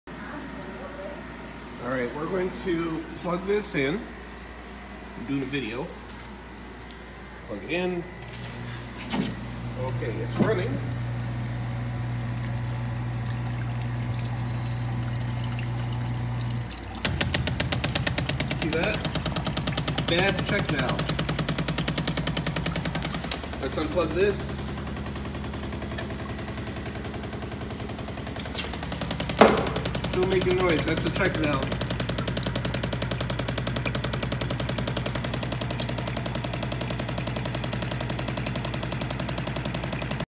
Weird Sump Check-valve failure
This checkvalve developed a failure that caused it to shake loose from the sump below and allow a fountain of water in the basement. When the pump would stop; the pressure above would leak down through the checkvalve and cause the flapper to “bang” at high frequency.

We fixed by simply replacing the component.